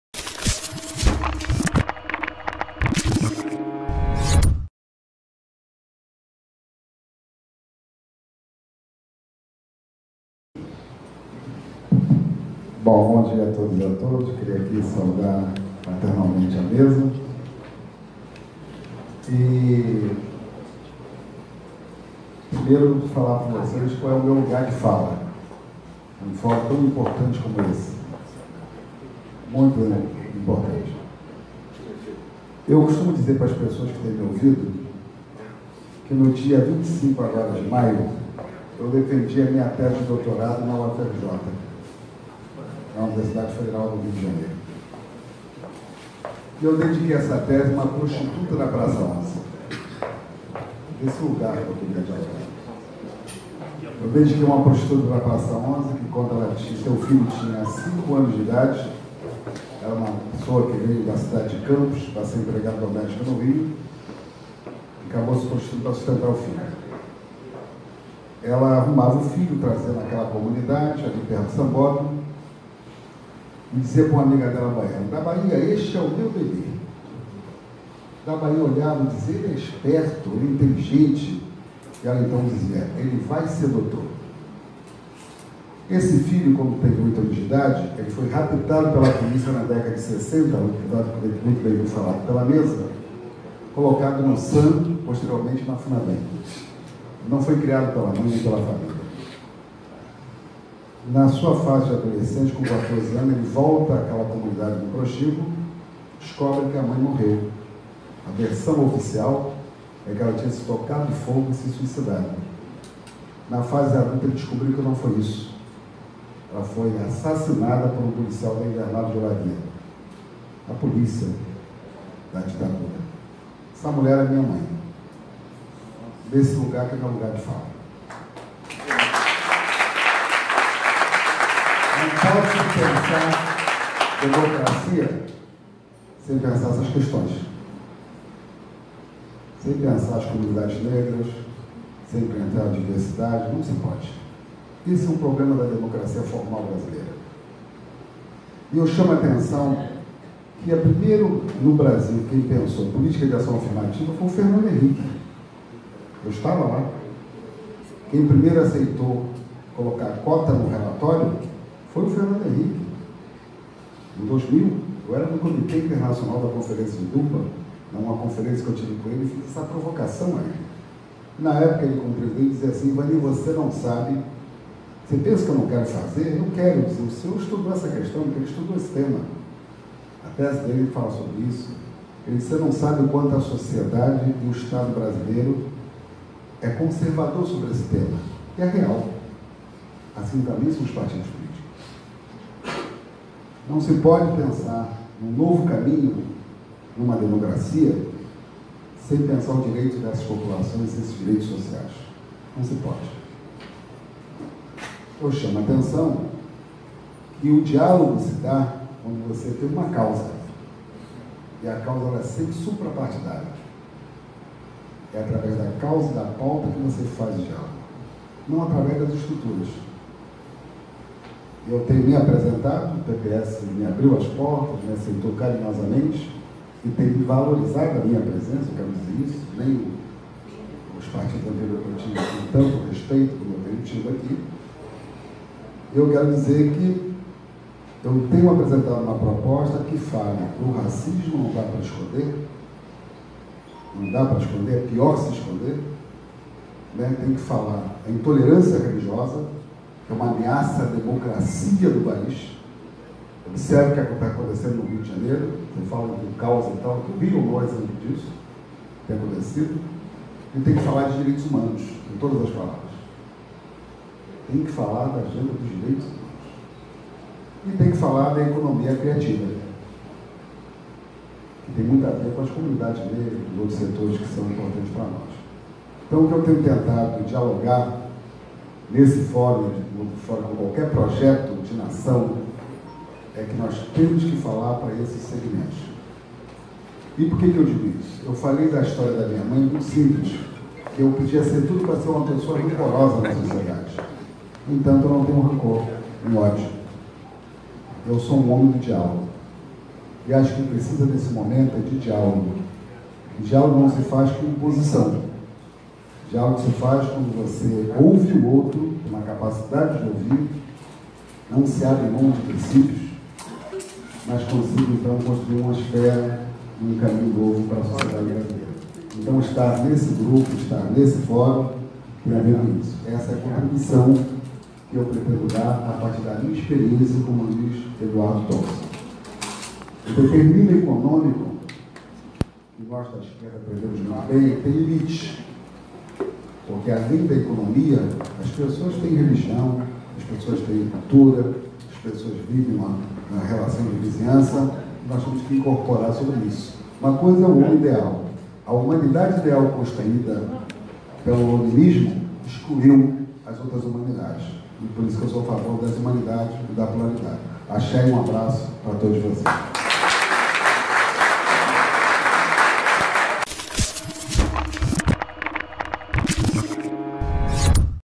Discurso de Ivanir do Santos, Babalawô e historiador, no Ato do Polo Democrático e Reformista
O evento aconteceu no RJ no último sábado (17), no auditório do Colégio dos Cirurgiões, Botafogo.

O Ato tem como objetivo a união do campo democrático brasileiro contra candidaturas